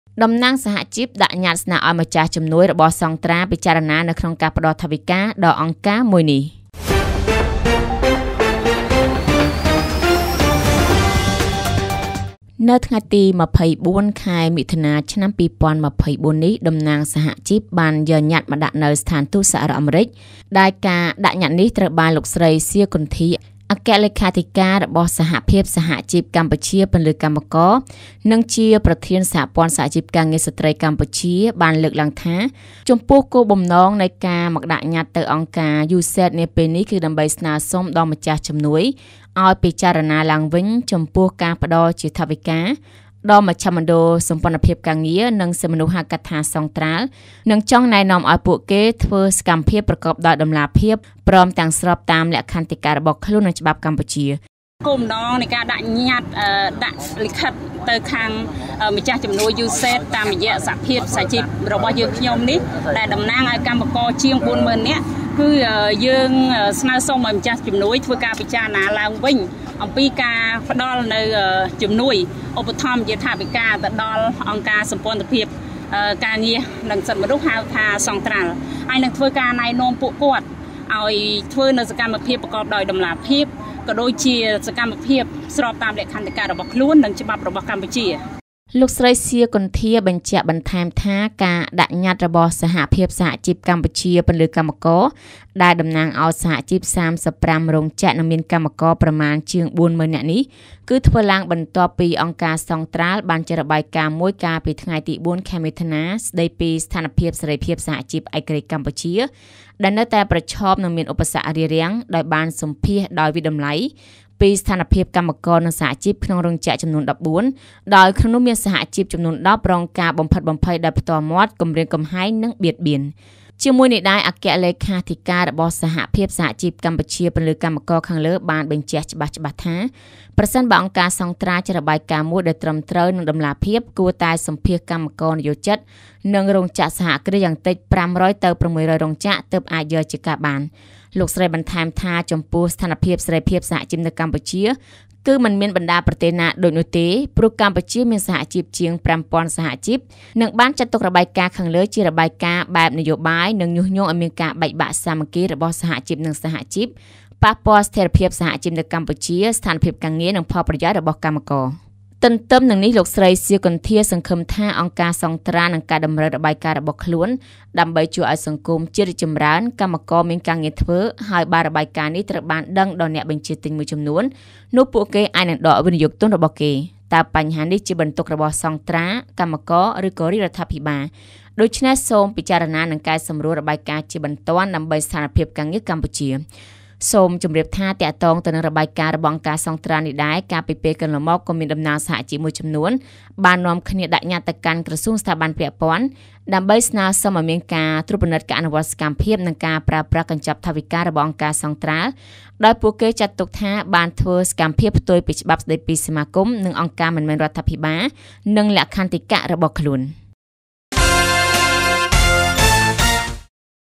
តំណាងសហជីពដាក់ញត្តិស្នើឱ្យម្ចាស់ជំនួយរបស់សង់ត្រាល់ពិចារណានៅក្នុងការផ្តល់ថវិក
លោកស្រី សៀ គន្ធា អគ្គលេខាធិការរបស់សហភាពសហជីពកម្ពុជាពន្លឺកម្មករ បានលើកឡើងថា ចំពោះគោលបំណងនៃការមកដាក់ញត្តិនៅស្ថានទូតអាមេរិកនាពេលនេះ គឺដើម្បីស្នើសុំដល់ម្ចាស់ជំនួយឱ្យពិចារណាឡើងវិញ ចំពោះការផ្តល់ជាថវិកា ដល់អង្គការសង់ត្រាល់ និងចង់ណែនាំឱ្យពួកគេធ្វើសកម្មភាពប្រកបដោយតម្លាភាព ព្រមទាំងស្របតាមលក្ខន្តិកៈរបស់ខ្លួននិងច្បាប់កម្ពុជា...